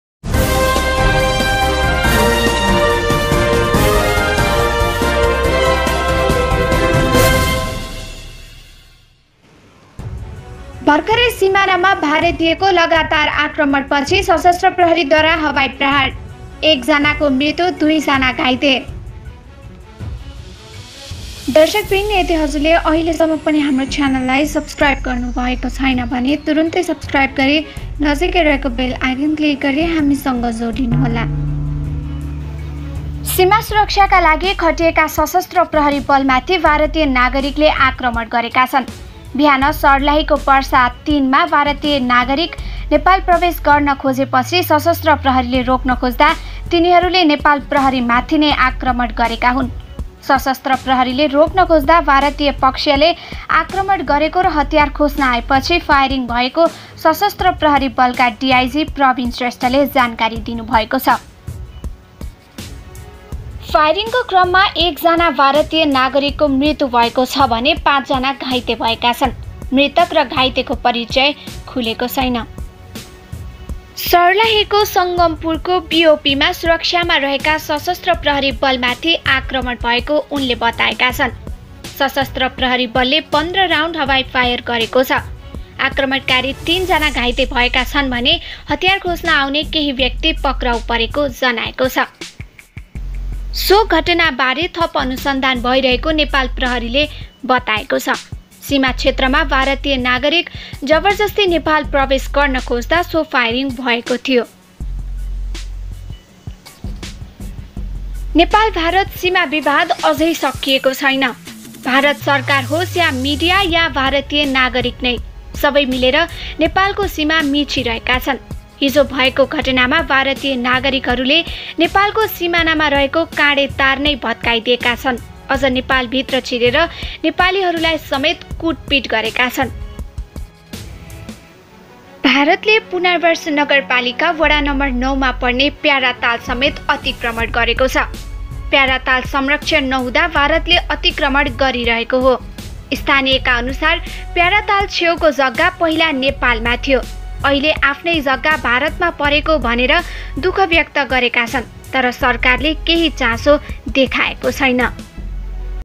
नेपाल भारत सीमा मा गोली चल्यो । १ को मृत्यु २ जना घाइते। Nepal India border news
Nepal and India border issue regarding kalapami lipulek .
watch Latest Nepali news live update of today on rapid khabar